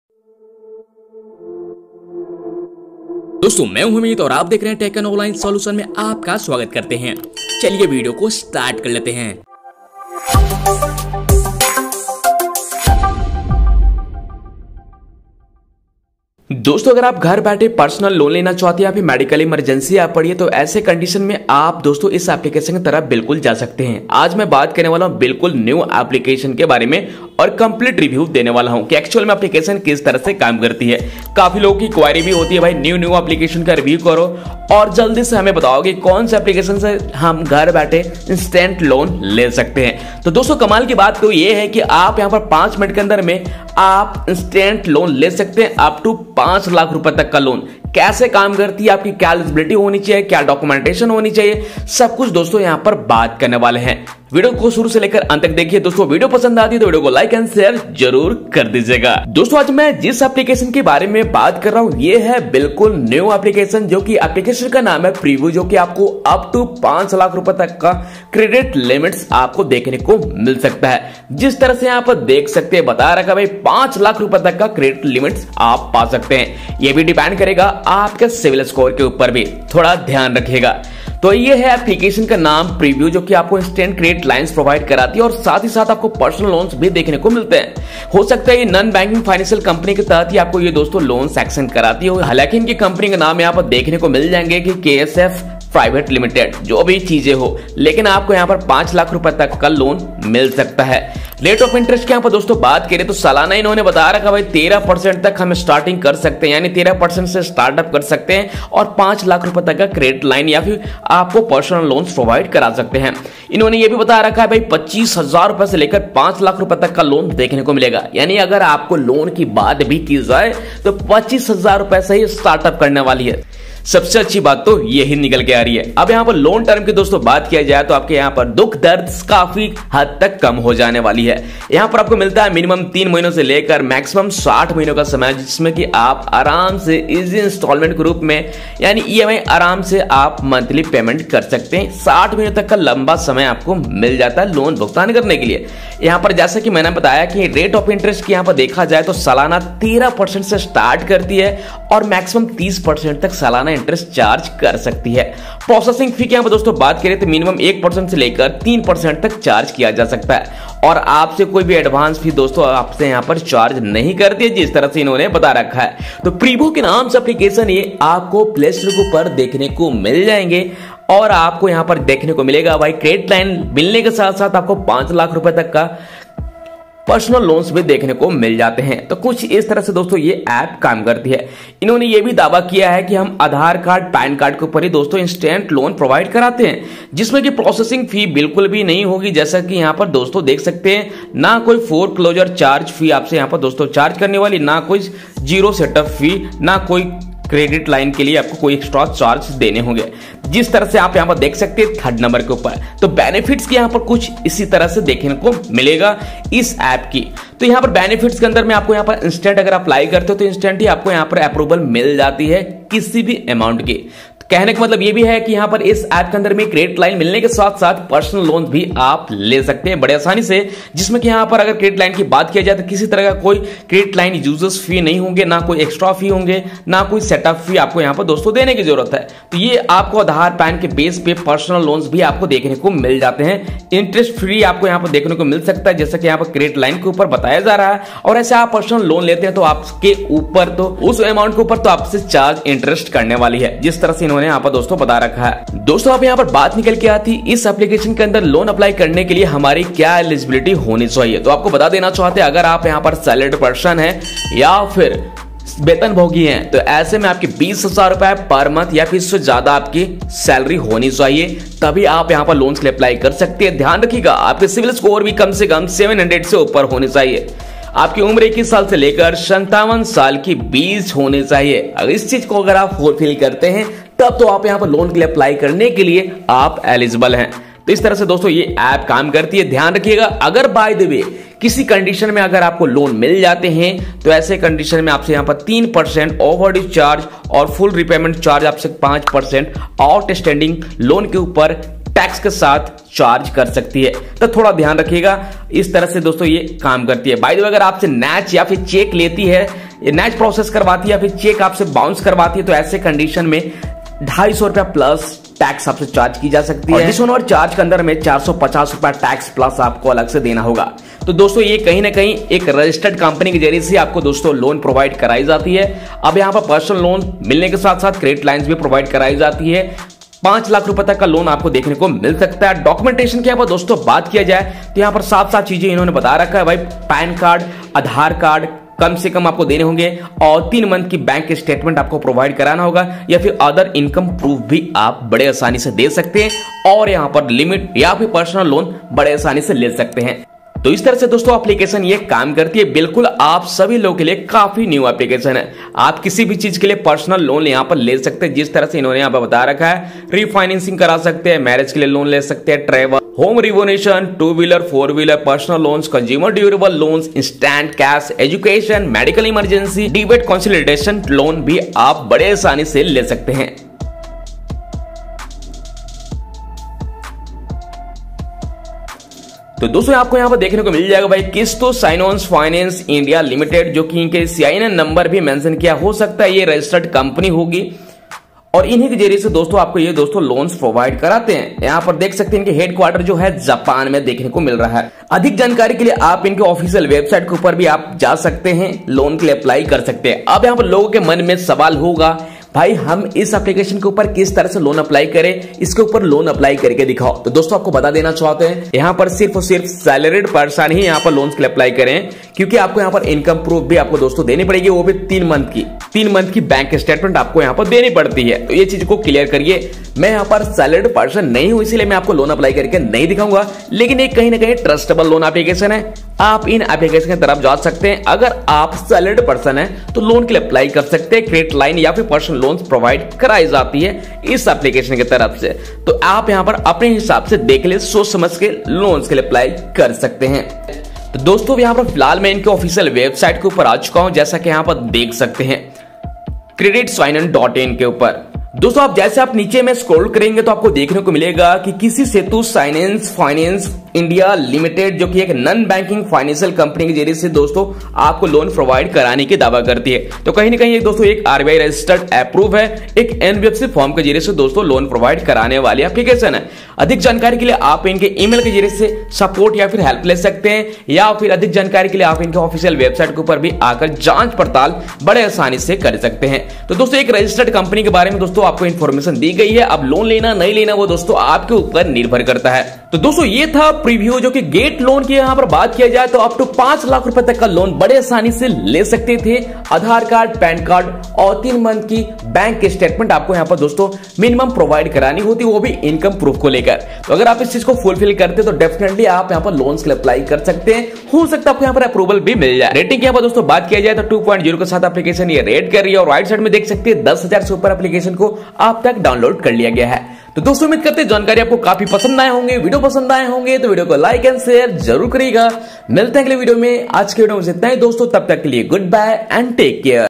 Privo: Get loan upto 5 lakhs | Instant Creditline & Personal loan app | Privo Loan App Reviews
Privo is an instant credit line and personal loan app from KSF Pvt Ltd, a leading Neo-Lending conglomerate, that lets you borrow money when you need it.

Pre-qualify in minutes and open a credit line up to 5 lakhs.

Interest rate starting from 13% p.a.